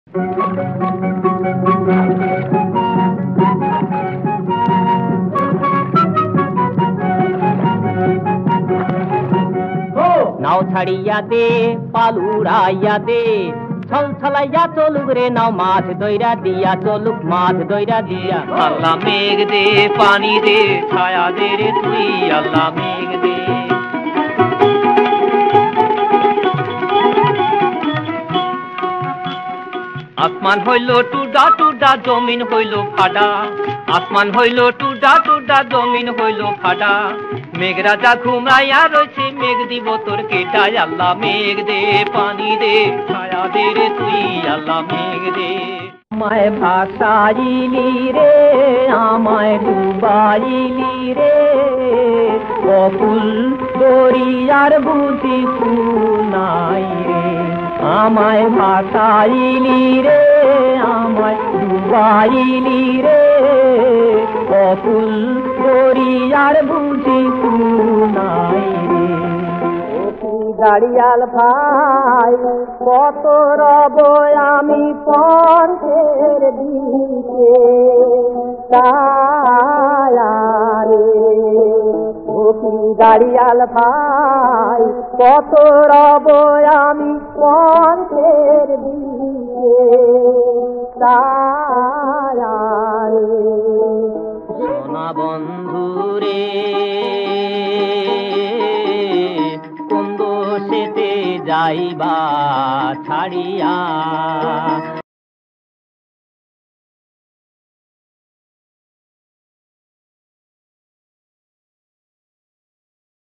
আব্বাসউদ্দীন আহমেদ l Bayanno TV
আব্বাসউদ্দীন আহমেদ ছিলেন একজন প্রখ্যাত বাংলা লোকসংগীত গায়ক, সুরকার ও পরিচালক